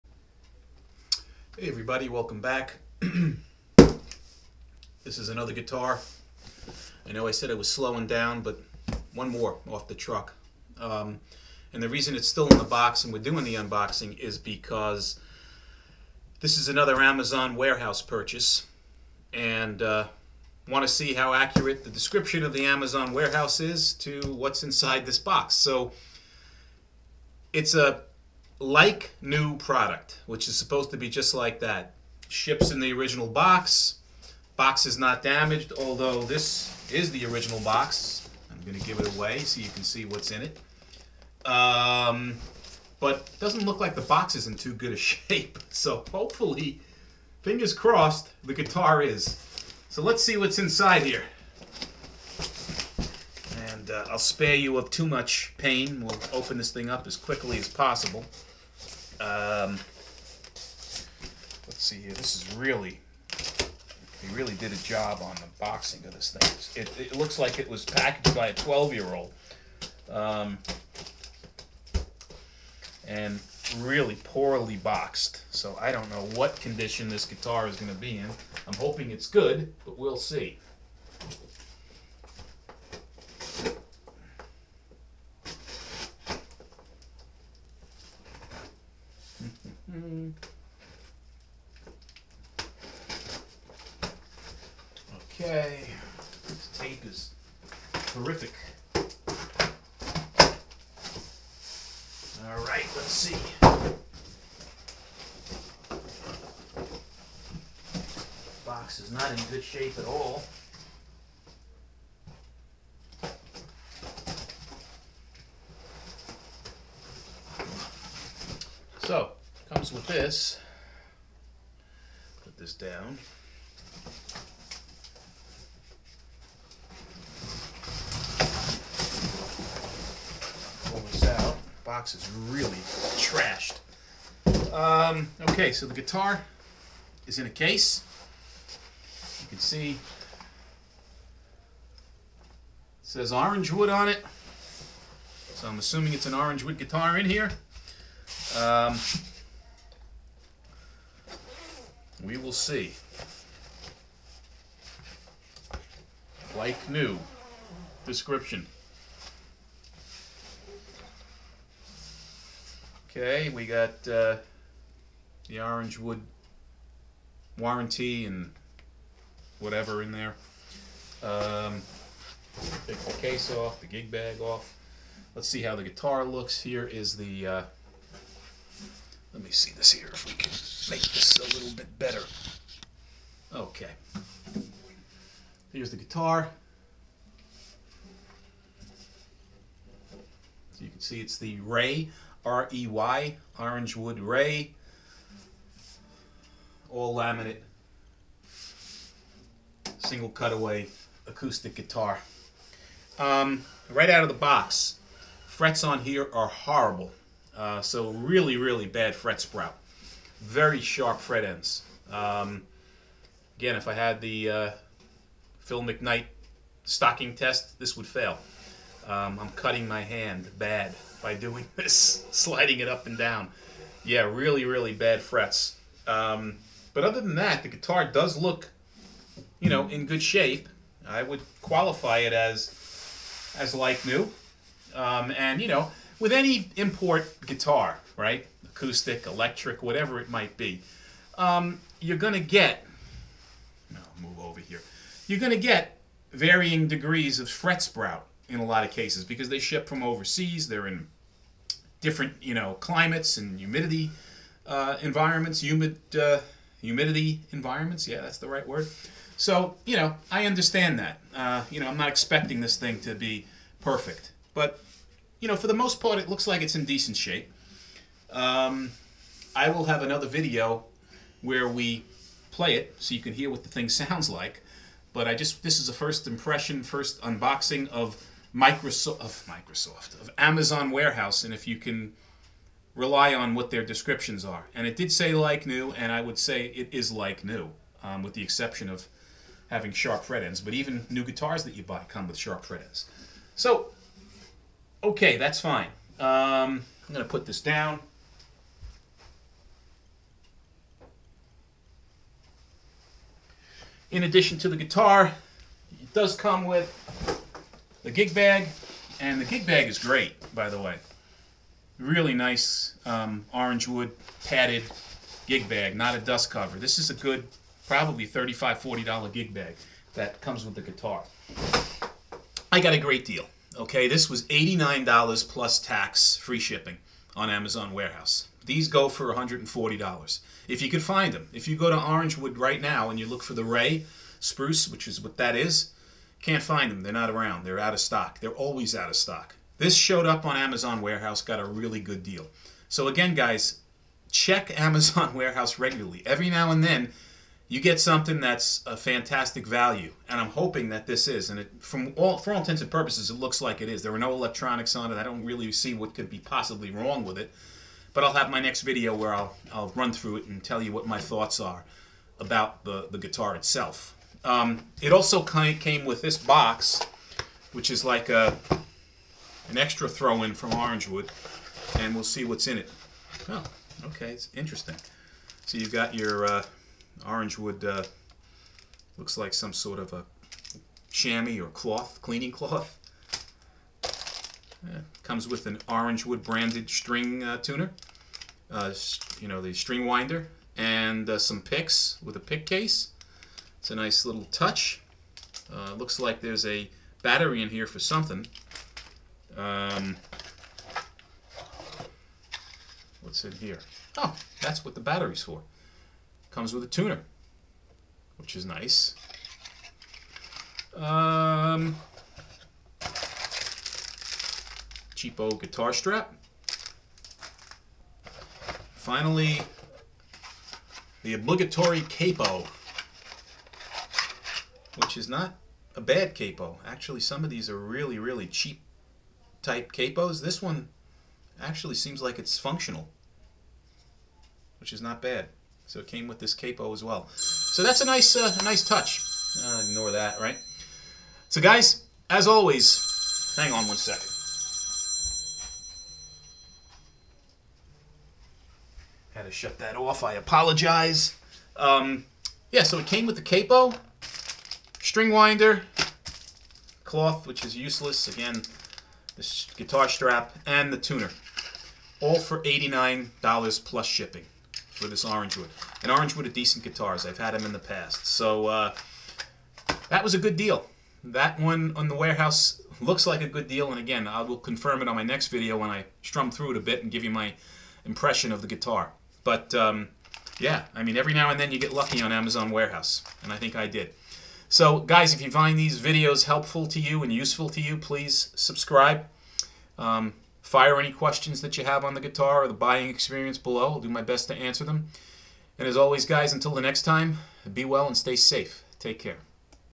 Amazon Warehouse Orangewood Rey Unboxing - Is It Really "Like New"?
What to expect from an Amazon Warehouse product described as "like new".